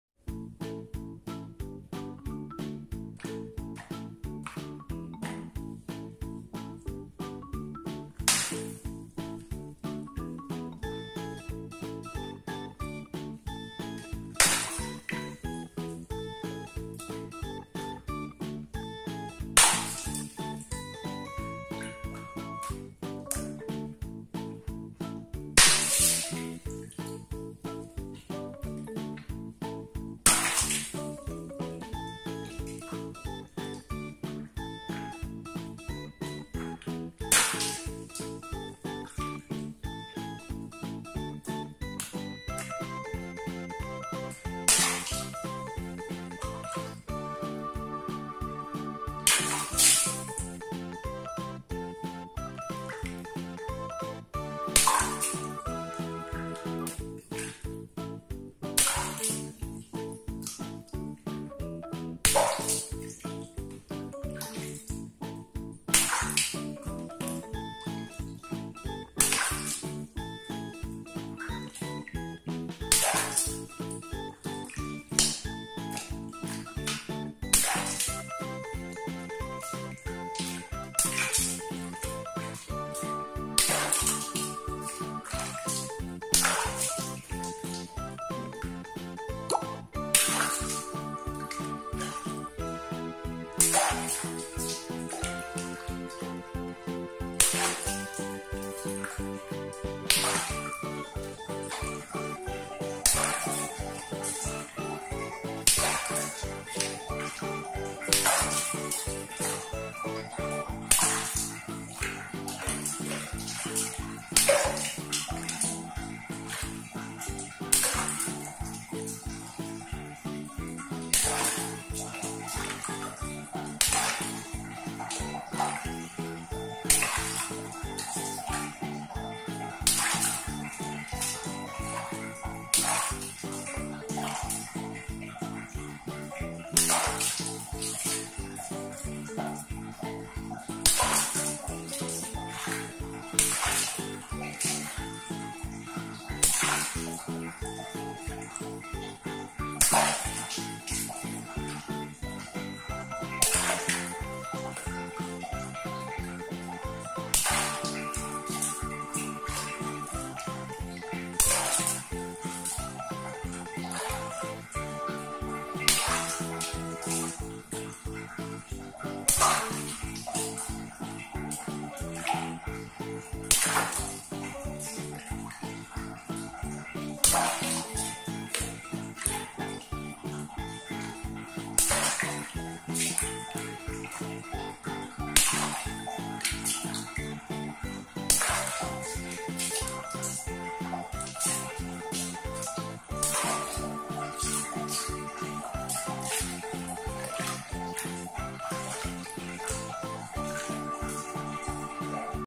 WATER BALLOONS POP!! FUN POPPING BALLOONS!! ASMR!! SATISFYING VIDEO!! (NO TALKING)
Bursting Balloons Water Balloons pop ASMR ! Satisfying sounds ! (no talking)

FUN BURSTING POLYTHENE BALLOONS
Welcome to popping only
Our aim is to provide audience relaxing sounds effects of water balloons popping to reduce stress !